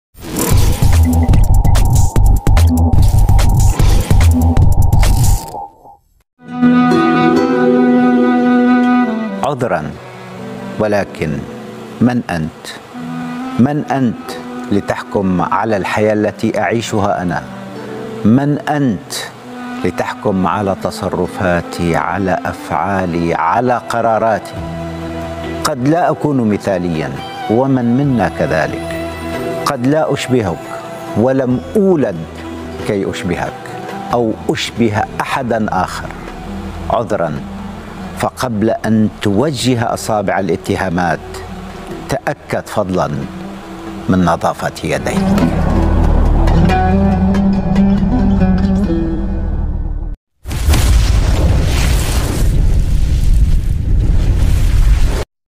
عذراً من أنت؟ لتحكم على الحياة التي اعيشها أنا ؟
عذراً
من أنت؟
لتحكم على
الحياة التي اعيشها أنا ؟
من أنت ؟
لتحكم على تصرفاتي
على أفعالي
على قراراتي ؟
قد لا أكون مثالياً
ومن منا كذلك
قد لا اشبهك
ولم أولد كي أشبهك
أو أشبه أحداً آخر
عذراً
فقبل
أن توجه أصابع الإتهامات
تأكد فضلاً
من نظافة يديك

جميع الحقوق محفوظة لـ قناة @MBCGROUPTv @mustafaalagha1